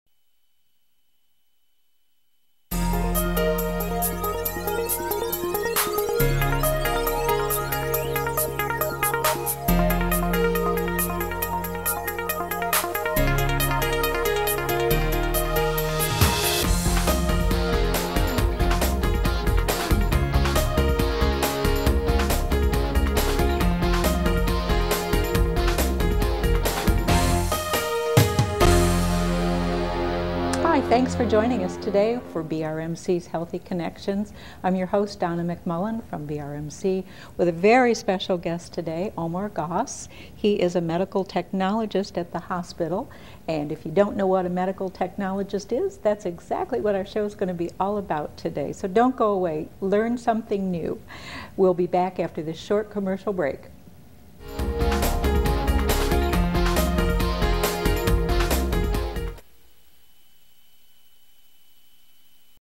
HC20130130_1.flv
Host Donna McMullen and special guest Omar Goss, Medical technologist at BRMC.